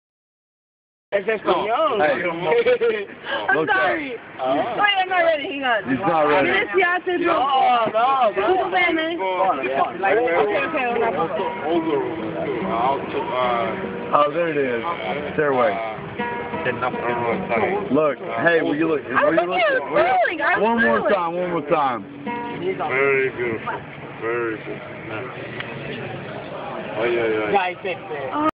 florence02
group of friends we met in florence Italy.